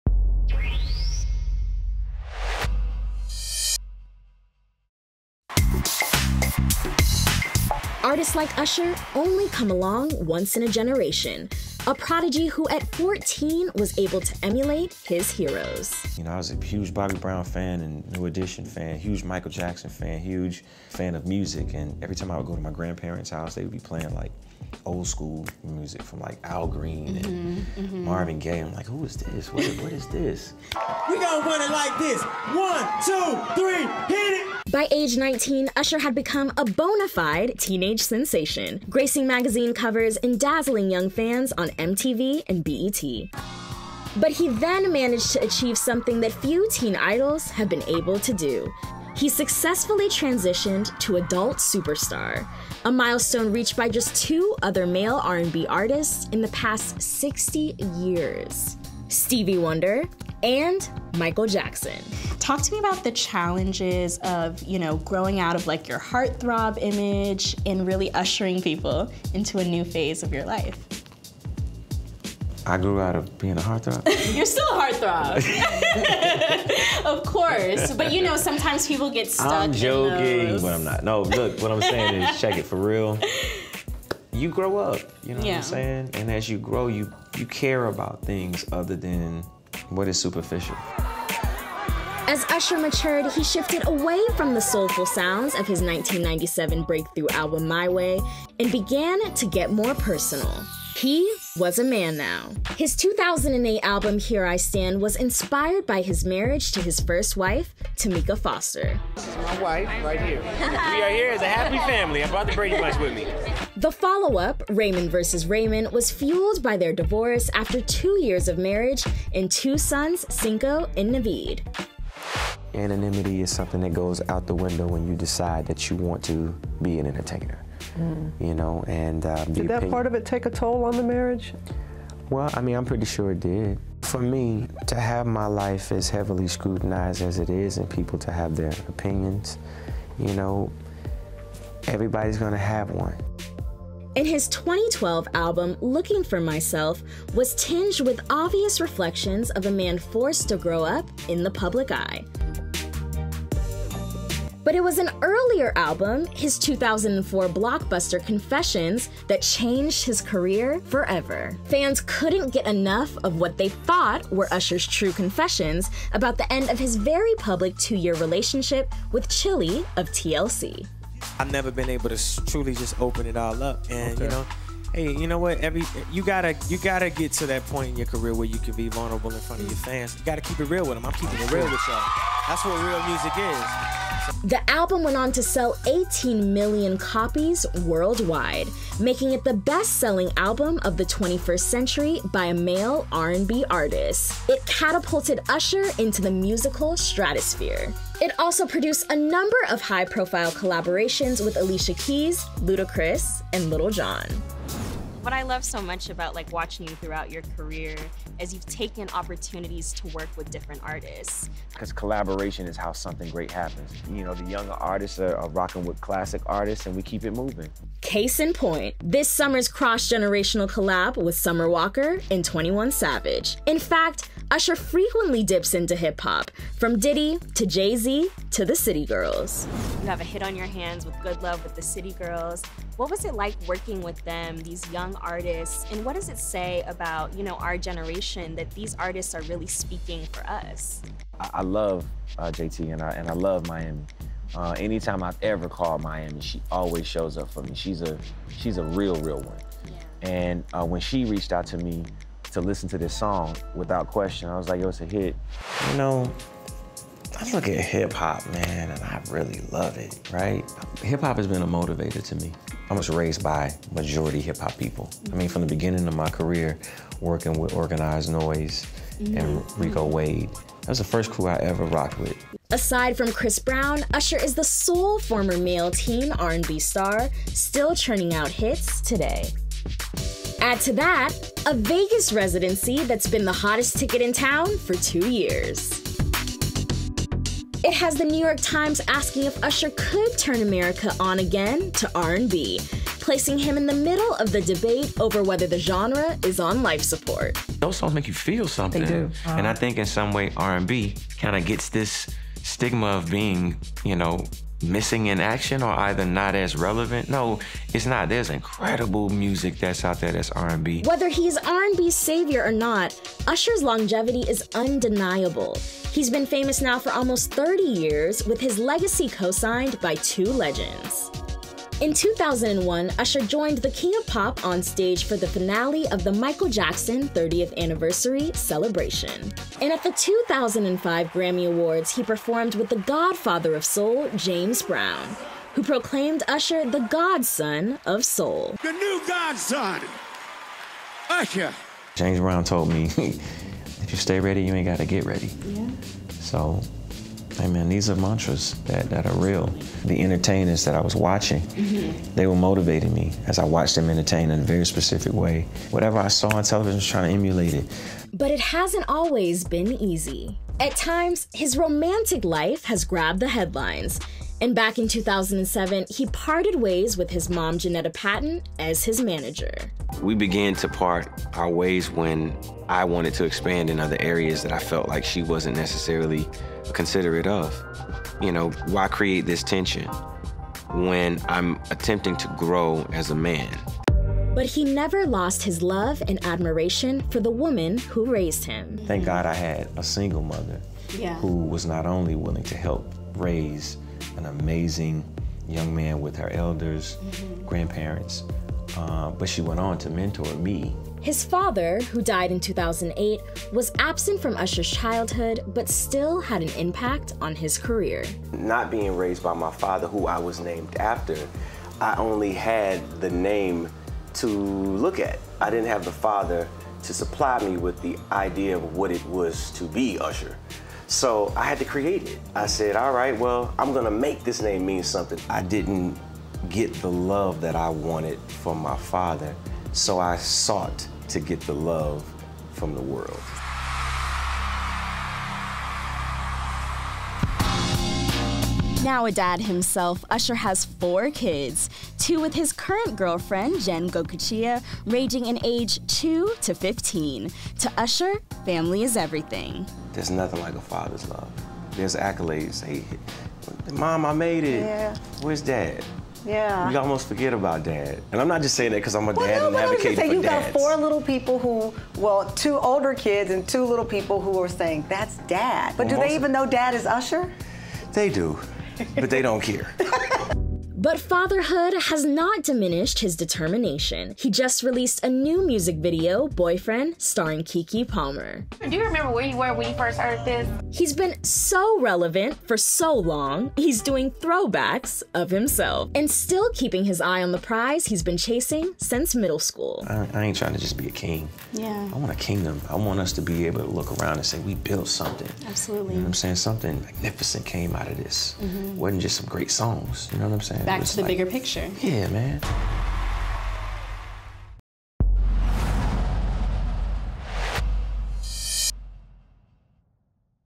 Usher Opens Up About Fatherhood, Divorce, and Carrying on the R&B Torch | REVOLT Black News
In this week's episode of REVOLT Black News Weekly, we sit down with R&B superstar Usher for an in-depth conversation about his prolific 30-year career. Usher reflects on his rise to fame as a teenager, becoming an R&B heartthrob, transitioning to more mature music as he grew older, going through a high-profile divorce, and embracing fatherhood. He also discusses carrying on the R&B torch handed to him by legends like James Brown and Michael Jackson, his Vegas residency, and collaborating with younger artists to keep the genre alive. Watch!

Text us! Someone from the squad will text you back.